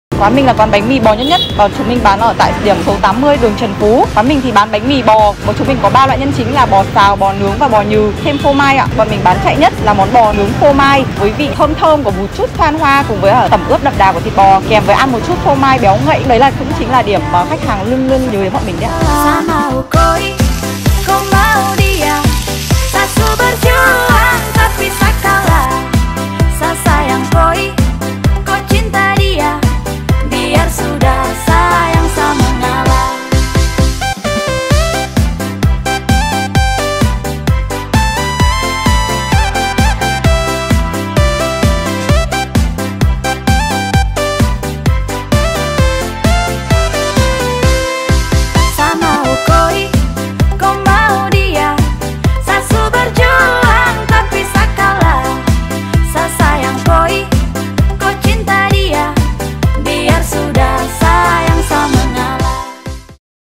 BÁNH MỲ BÒ PHÔ MAI CỨ PHẢI THẾ NÀY MỚI ĐÃ - Món Ngon Hải Phòng
Phô mai phủ phê kéo dài miên man, thịt bò 2 loại nướng, xào và nhừ phù hợp với khâu vị của từng bạn 😋
Một chiếc bánh mỳ xinh hội tụ đầy đủ chất dinh dưỡng cho 1 cảm xúc dân trào khi ăn : ))
Hiện tại quán bán mang về nhé
Nhất Nhất - 80 Trần Phú, Ngô Quyền, Hải Phòng (14h30 - 22h)